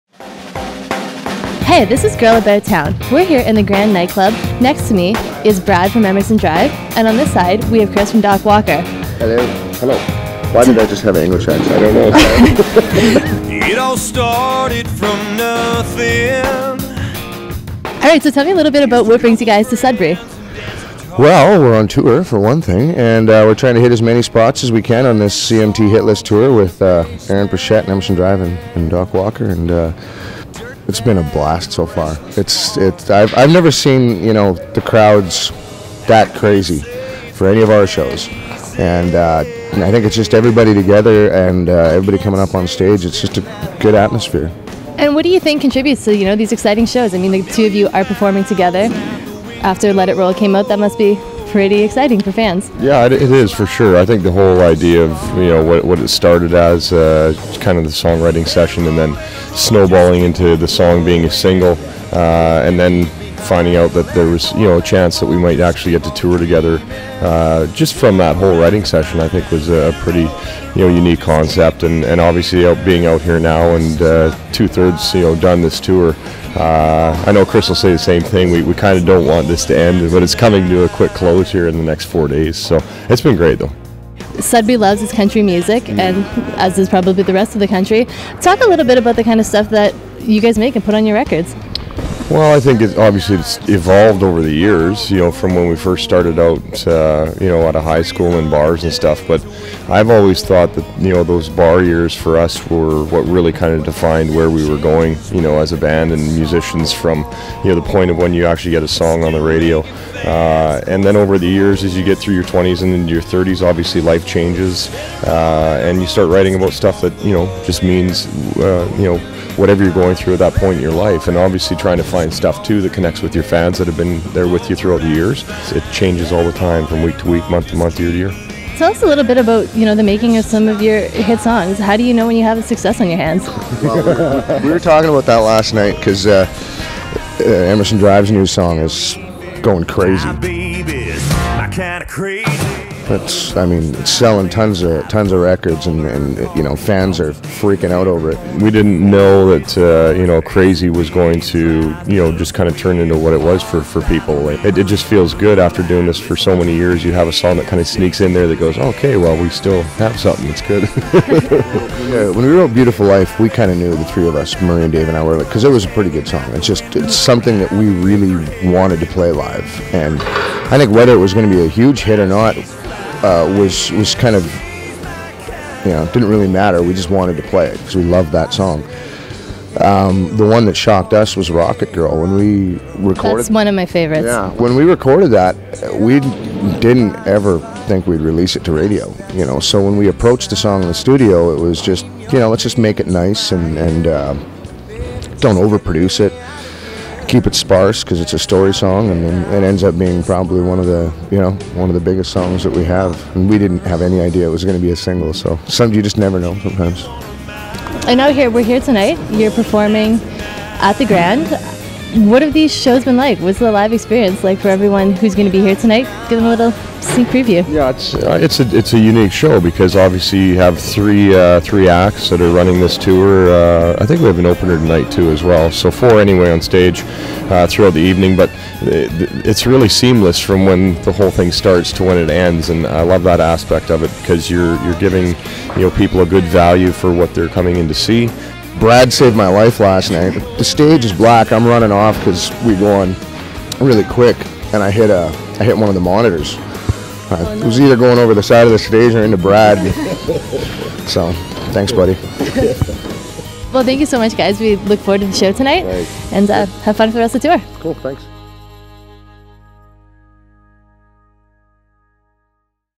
Emerson Drive & Doc Walker collide in Sudbury
Tickets for the CMT Hitlist Concert sold out quickly in Sudbury. Brad Mates' of Emerson Drive and Chris Thorsteinson from Doc Walker sat down with Northern Life's Girl About Town to talk about the show.